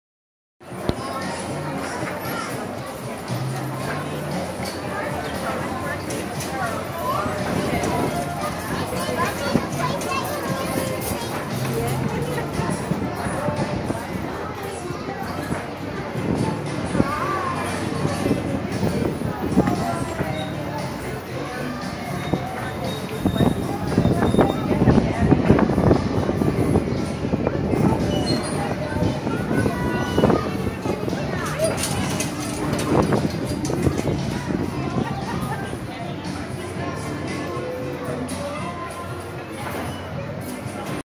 Leo rides the carousel
He did not like it that much!